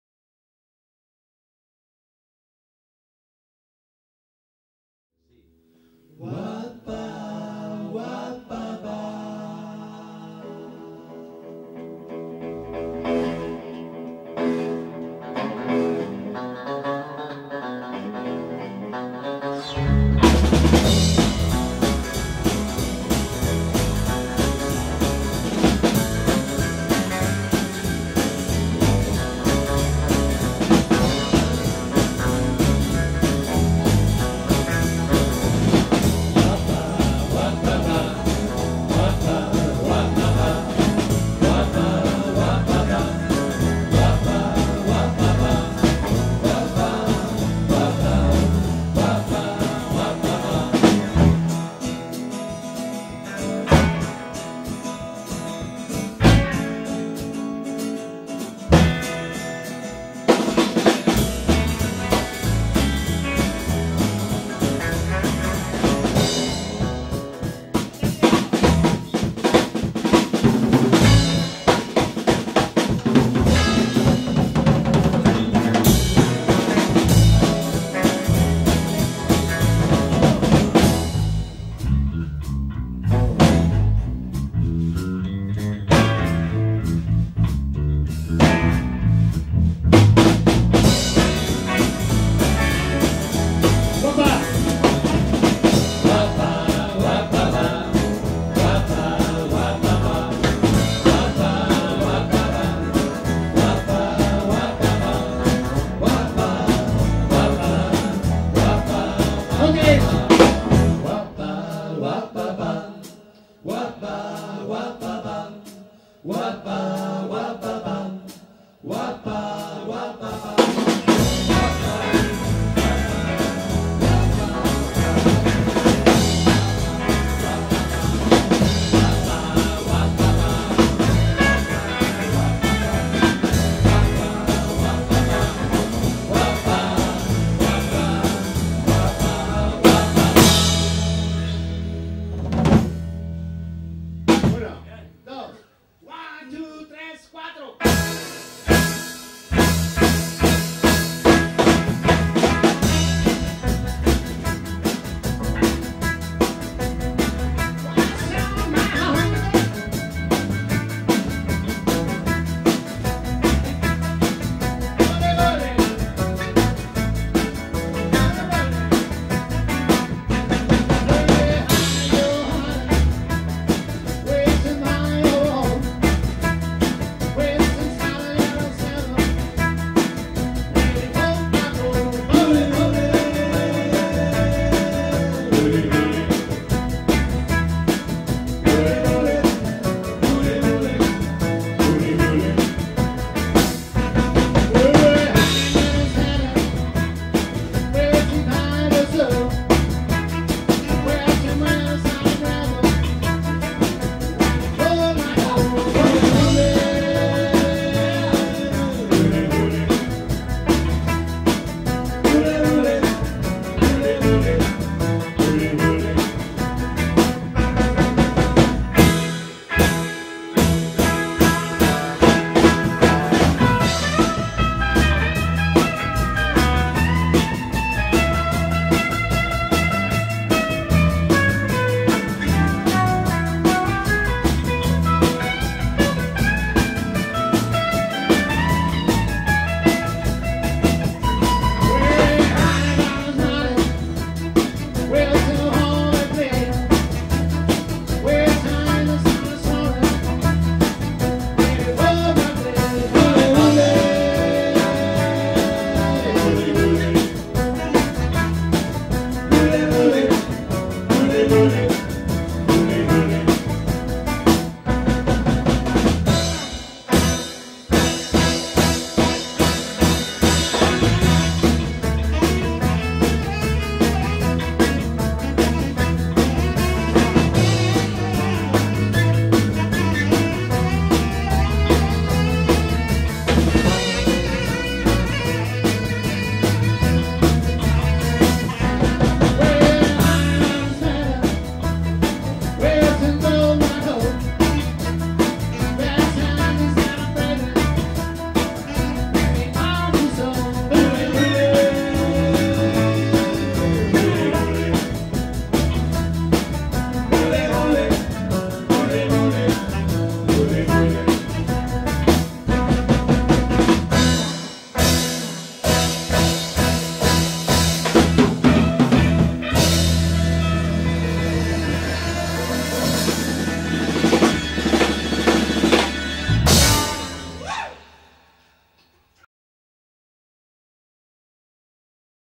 Tiana Club Band (Ensayo, 2003)
La banda Tiana Club Band en un ensayo (Con fallos)grabado en julio de 2003